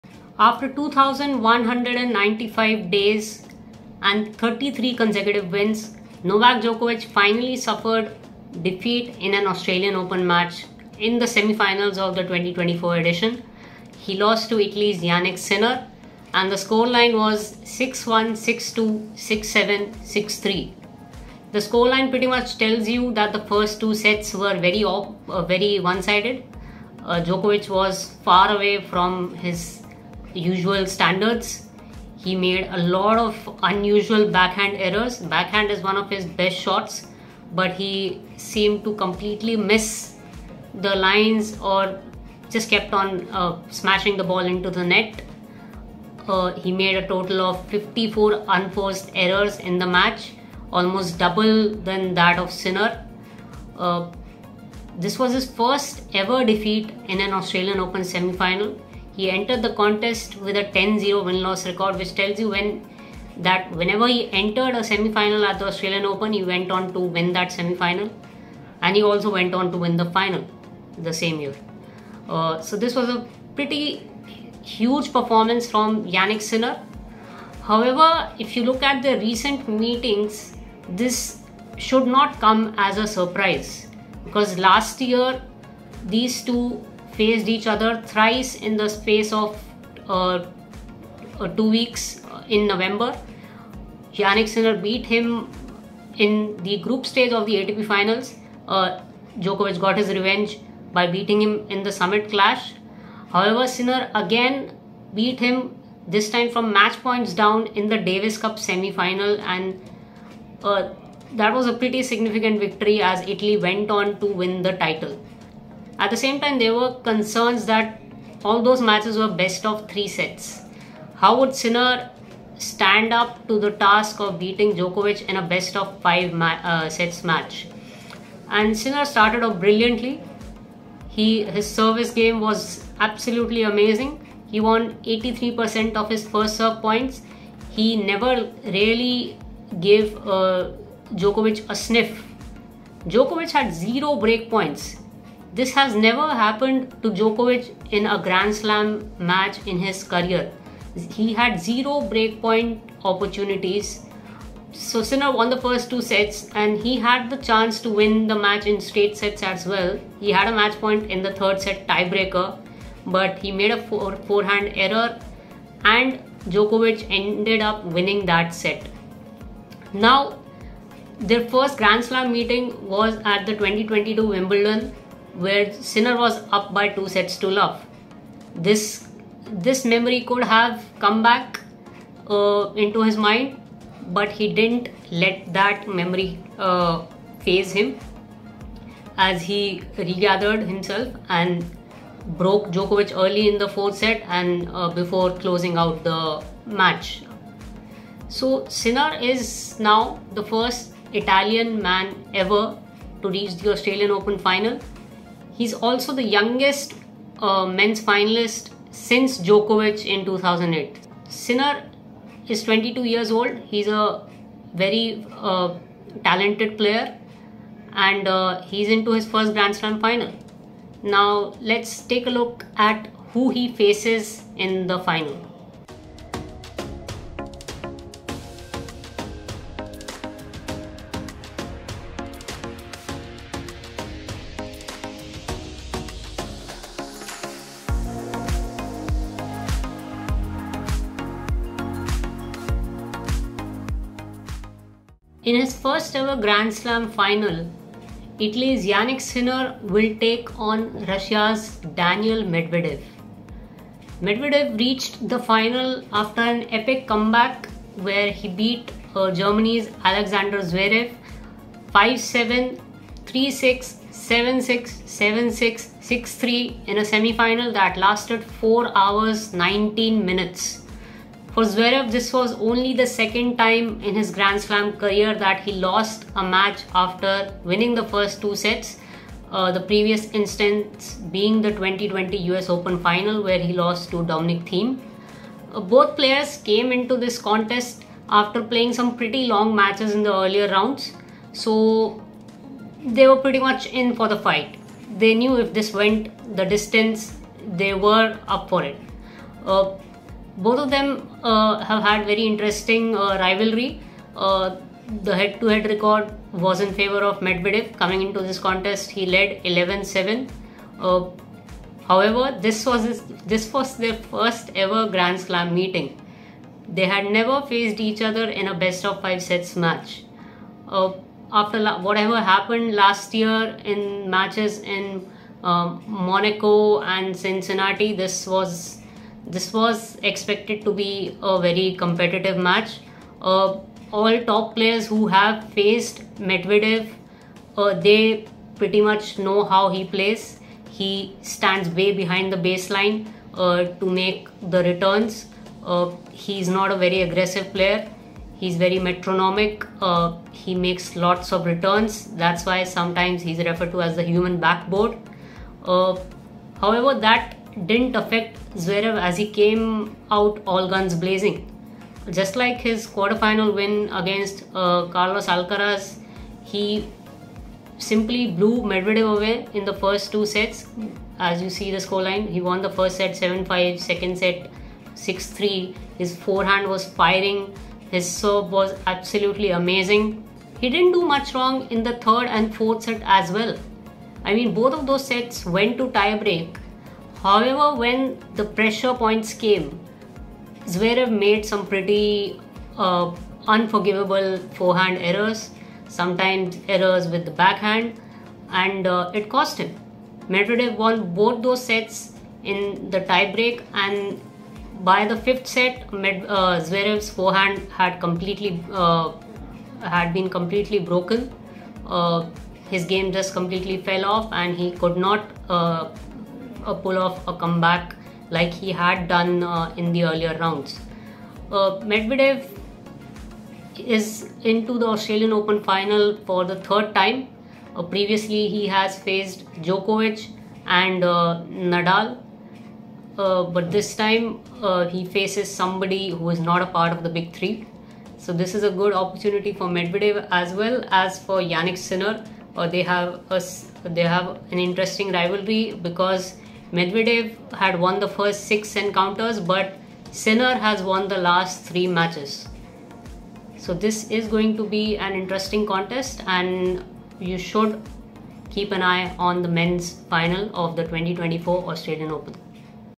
Australian Open final - Medvedev vs Sinner - how we got here, milestone check and more
A new men’s champion will be crowned at Melbourne Park on Sunday night when two-time Australian Open runner-up Daniil Medvedev and first-time finalist Jannik Sinner square off for the right to be the last man standing.